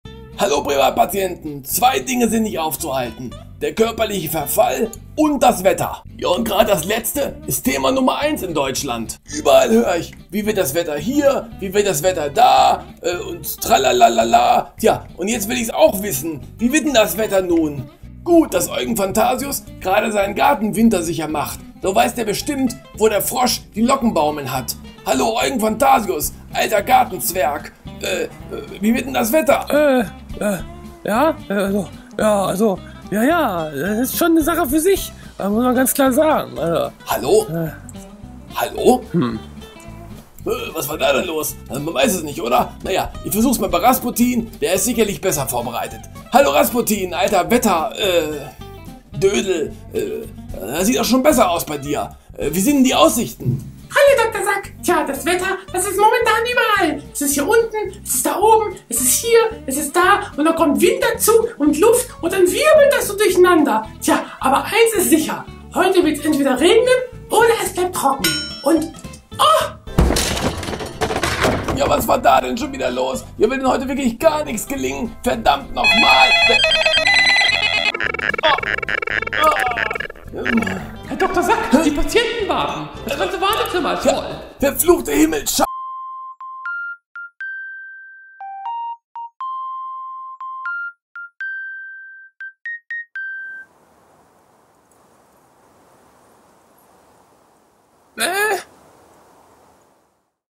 Die Wettervorhersage | Quickies
Dr. Sack wird schon ganz verrückt: Überall hört er nur WETTER, WETTER, WETTER. Und so kommt es, dass auch er nun wissen möchte, wie das Wetter wird.

Doch das ist schwieriger, als gedacht. Hätte er mal Jörg Kachelmann gefragt!

COPYRIGHT
Henning Plückebaum